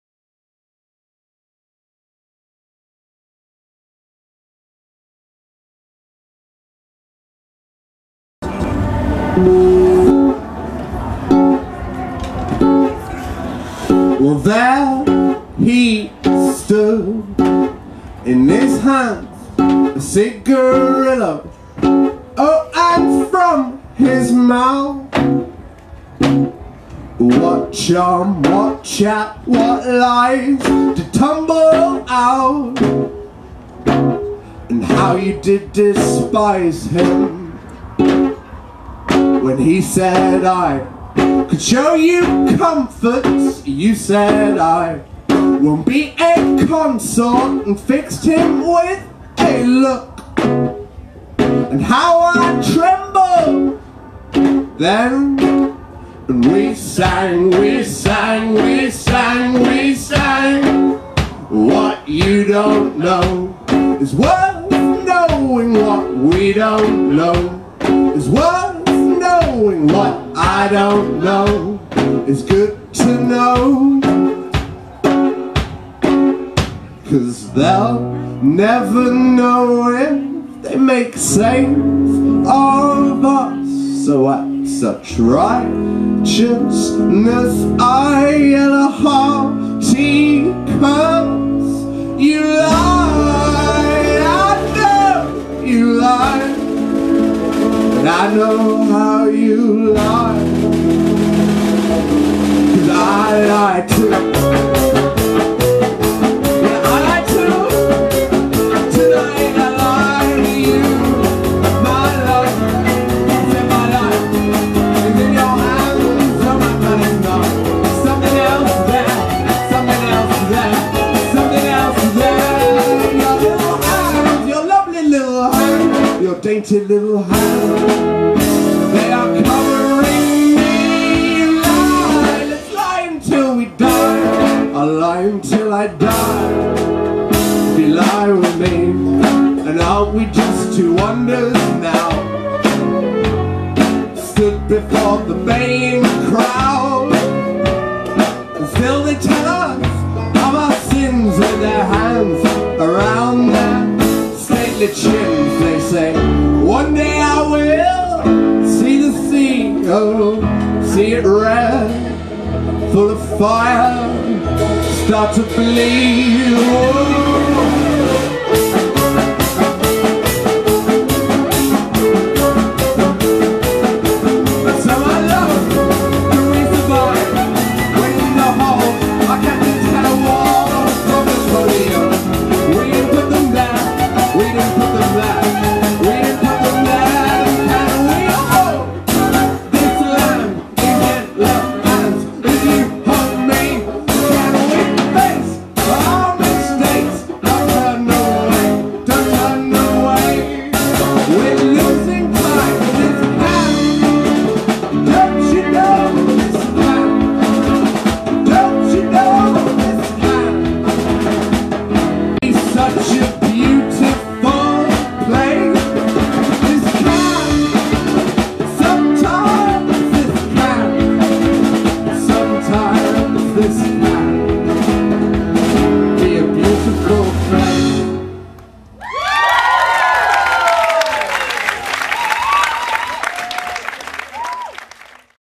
FIREWORKS NIGHT ON THE LOCAL STAGE @ EOTR 2007
Fireworks Night performing Live on The Local stage at The End Of The Road festival 2007